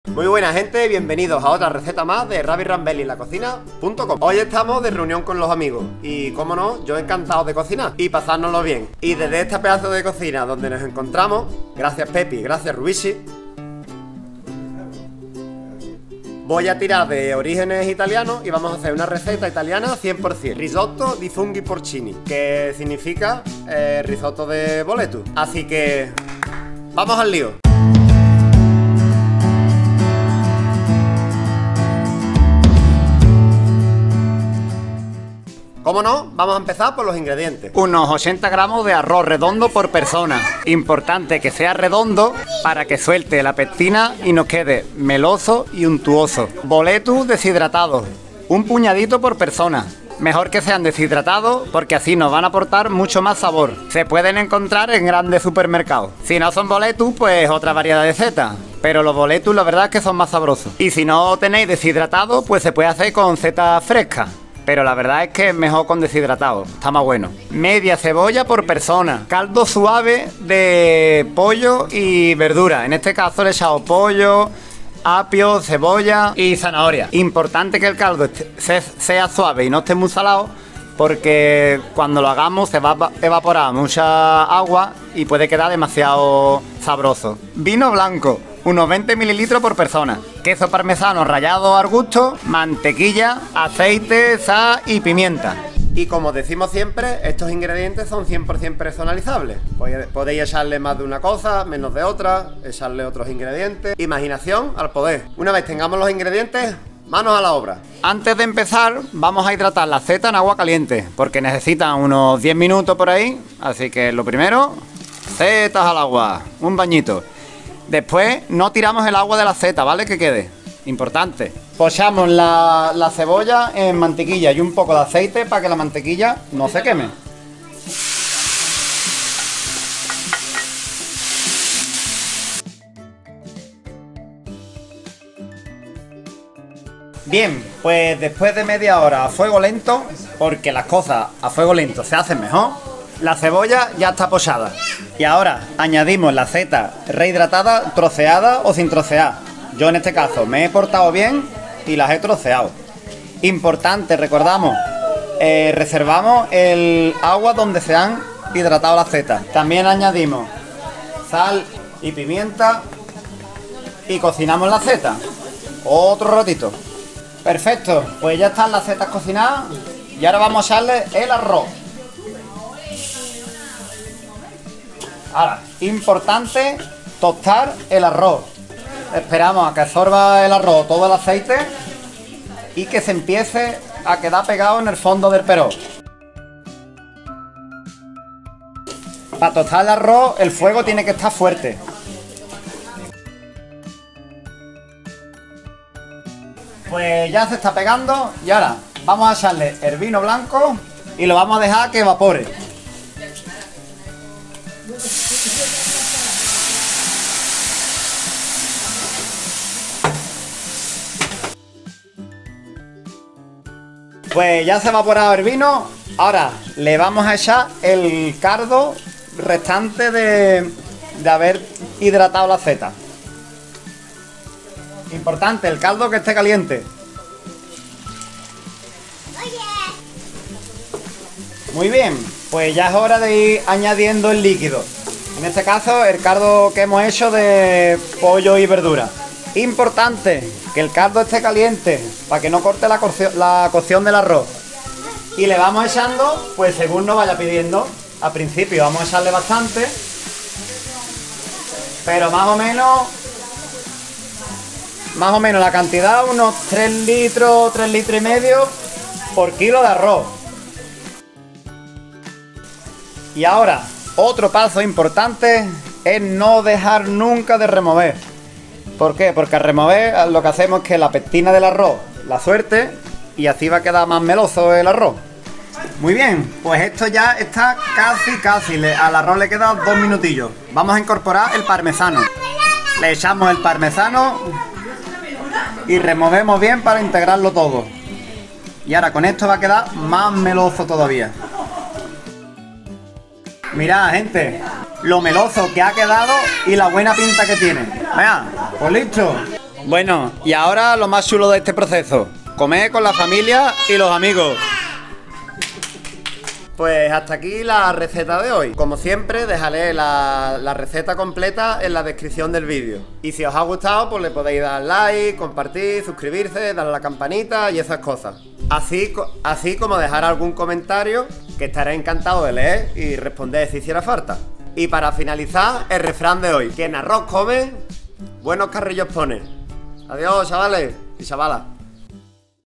RISOTTO DE SETAS (BOLETUS DESHIDRATADOS) RECETA ITALIANA
👇 👇 Receta completa aquí!!👇 👇
Muy buenas de nuevo familia y bienvenidos a otra receta de ravirambellienlacocina.com
Hoy voy a tirar de orígenes italianos y vamos a hacer una receta italiana 100 %: risotto di funghi porcini, que significa risotto de boletus. Vamos a utilizar unos boletus deshidratados, ya que con este tipo de seta el arroz coge mucho más sabor. Este tipo de setas deshidratadas las podéis encontrar en las grandes superficies sin dificultad. No tienen por qué ser obligatoriamente boletus, sino que vale cualquier tipo de seta que tenga sabor. Y si no teneís setas deshidratadas pues valen las setas frescas también, eso sí, como dijimos antes mejor elegir una seta con sabor.
Con esta receta vamos a obtener un risotto muy meloso y sobre todo riquísimo. Ideal para disfrutarlo en reuniones familiares y de amigos.
INGREDIENTES:
- 80 gr de arroz redondo por persona. Importante que sea redondo para que suelte pectina y nos quede meloso.
- Boletus deshidratados. Un puñadito por persona. Como dijimos antes mejor que sean deshidratados ya que nos aportarán mucho más sabor.
- 1/2 cebolla picada por persona
- Caldo suave de ave y verduras. 2 PARTES Y MEDIA DE CALDO POR CADA PARTE DE ARROZ (aunque siempre es mejor reservar un poco más por si nos quedamos cortos). Yo en este caso he hecho un caldo simple de pollo con apio, zanahorias y cebolla. Importante que no esté muy salado porque el líquido evapora y se concentra más el sabor
- Vino blanco
- Queso parmesano rallado
- Mantequilla
- Aceite
- Sal
- Pimienta

Y como decimos siempre estos ingredientes son totalmente personalizables. podéis echar más de una cosa o menos de otra o echar otros ingredientes. imaginación al poder!!

MANOS A LA OBRA:
- Empezamos por hidratar las setas en agua caliente unos 10 min. Sacarlas y trocearlas al gusto y reservar el agua.
- Pochar la cebolla en mantequilla con un poco de aceite para que no se queme a fuego lento.
- Cuando la cebolla ya esté pochada añadimos las setas rehidratadas troceadas o sin trocear, yo me he portado y las he troceado. Añadimos sal y pimienta y cocinamos las setas
- Echar el arroz y tostarlo. Lo vamos moviendo hasta que veamos que se está quedando pegado al fondo.
- Cuando veamos que el arroz se empieza a pegar al fondo agregamos el vino y esperamos a que se evapore.
- Una vez se haya evaporado el alcohol del vino echamos el agua de las setas caliente que va a dar color y sabor.
- Y ya es la hora de ir añadiendo el líquido, en este caso el caldo. importante que esté caliente para que no corte la cocción. El caldo lo iremos echando poco a poco según vaya pidiendo el arroz, según se vaya gastando el líquido.
- TRUCO: no dejar de remover para que el arroz suelte la pectina y quede con una textura melosa.
- Una vez que el arroz esté casi a punto echamos el parmesano y removemos bien para integrarlo.
- Apartamos del fuego y lo dejamos reposar unos 3 o 4 minutos.

Y ya tenemos nuestro risotto italiano. Ya lo que nos queda es hincarle el diente!!
BUEN PROVECHO!!